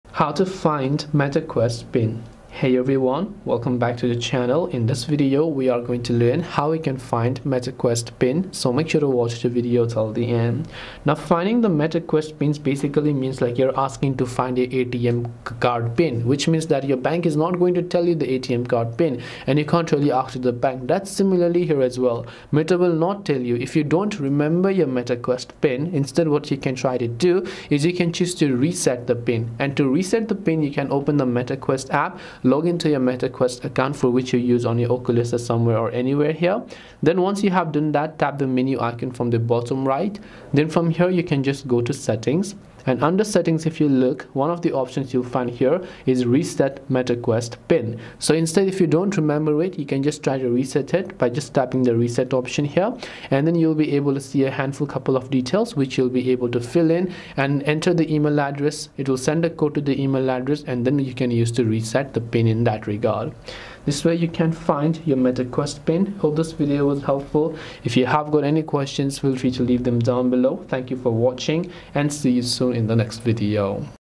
How to Find Meta Quest PIN
In this tutorial video, I will quickly guide you on how you can find your Meta Quest pin. So, make sure to watch this video till the end. We try to be responsive to as many questions as possible.

► Chapters:
0:00 Introduction
0:10 General Info
0:36 Find Meta Quest PIN
1:07 Conclusion